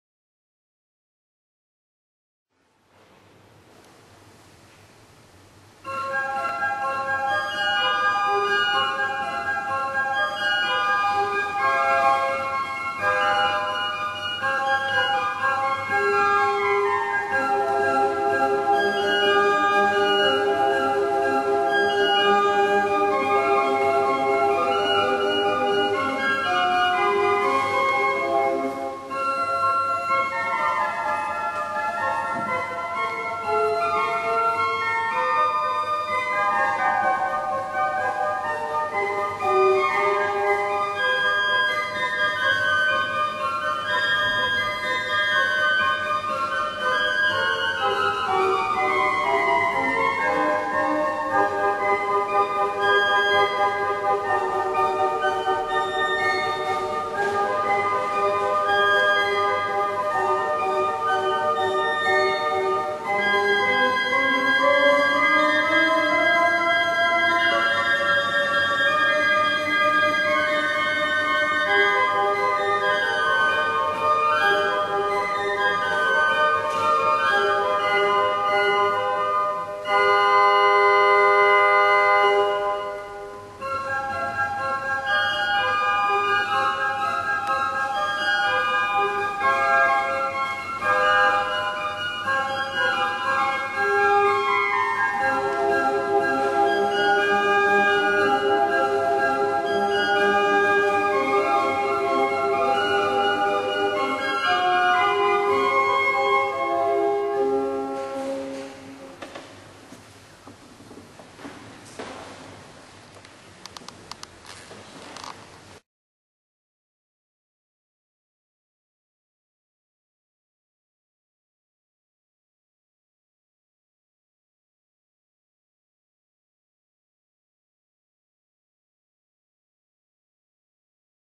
11° Rassegna Organistica dell'Isola Bergamasca 9 2011 a Grignano 511.wmv
Associzione Culturale  Promoisola  in Concerto per  organo  a Grignano di Brembate del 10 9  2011   organista  m°Carlo Benatti